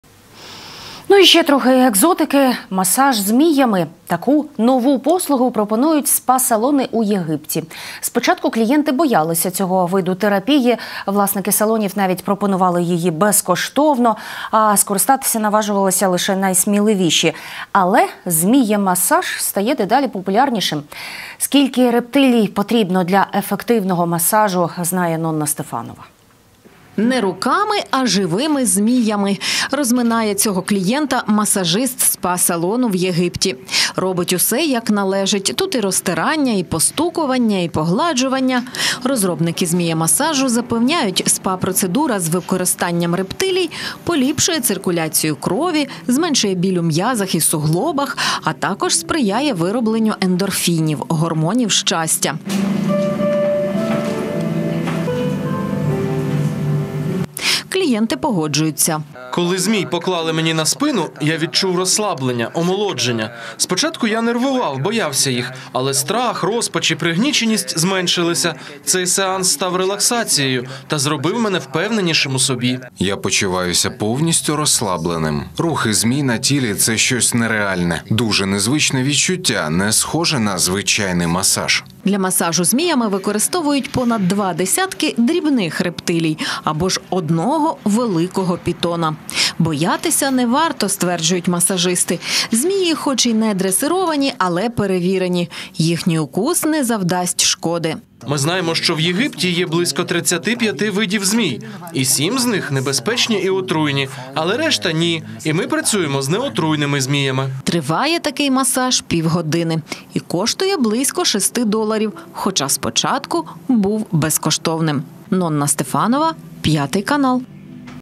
Екзотичний масаж зміями: Про незвичайну SPA-процедуру в салонах Єгипту
Масаж зміями. Таку нову послугу пропонують спа-салони у Єгипті. Спочатку клієнти боялися цього виду терапії. Власники салонів навіть пропонували її безкоштовно. А скористатися наважувалися лише найсміливіші. Але зміємасаж стає дедалі популярнішим. Скільки рептилій потрібно для ефективного масажу, знає Нонна Стефанова.